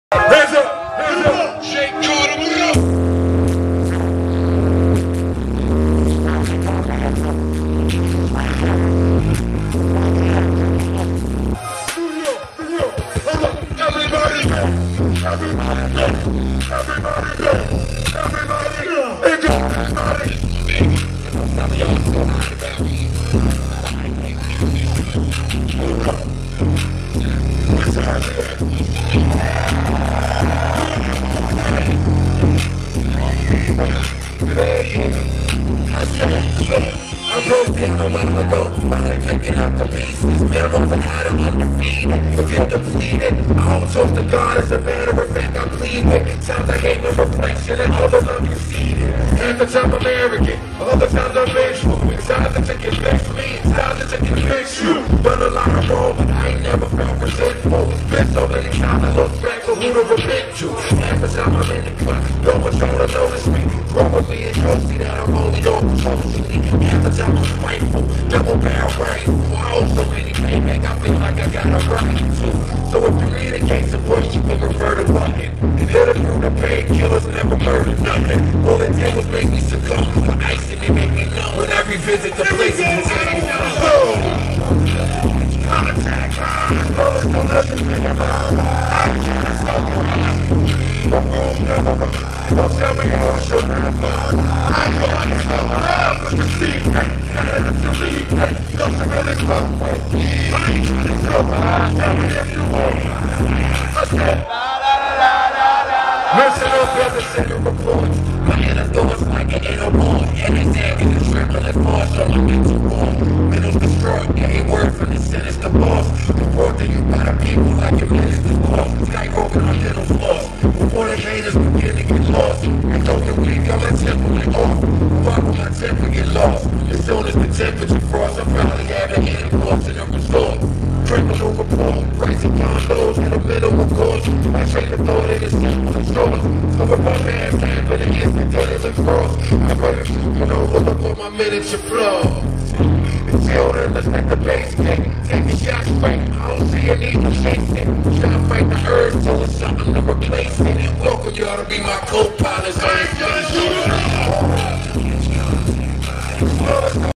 Joell Ortiz Live @RockTheBells 2011
Joey doing a new track at Rock The Bells 2011